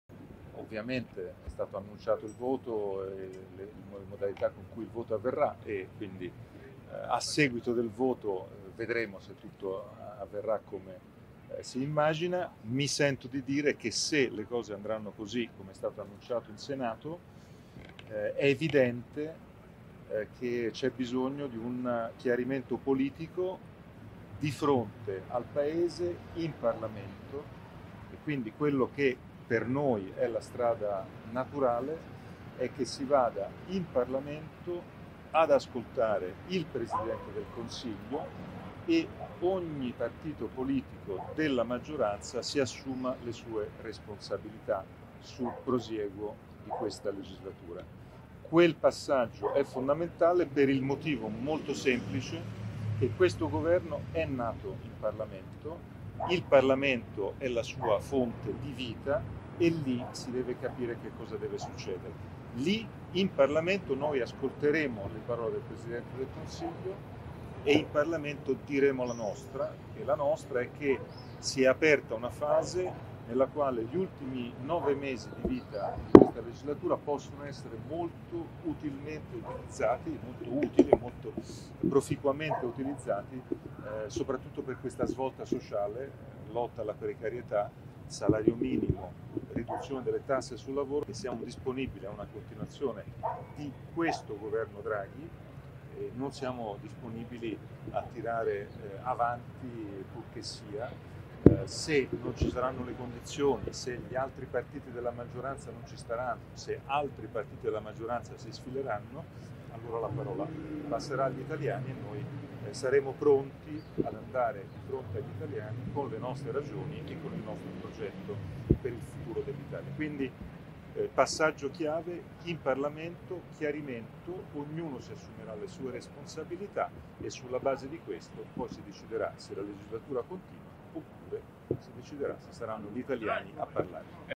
Governo, Letta: "Avanti con Draghi, altrimenti parola agli italiani"
Il leader del Pd: "Non siamo disponibili a tirare avanti purchessia"

POLITICA (Milano). "In Parlamento diremo che siamo disponibili a una continuazione di questo Governo Draghi, non siamo disponibili a tirare avanti purchessia". Così il leader del Pd, Enrico Letta, durante un punto stampa a Milano. "Se non ci saranno le condizioni, se altri partiti della maggioranza si sfileranno, allora la parola passerà agli italiani e noi saremo pronti ad andare di fronte agli italiani con il nostro progetto per il futuro dell'Italia", ha aggiunto. (Alessandro Boldrini/alanews)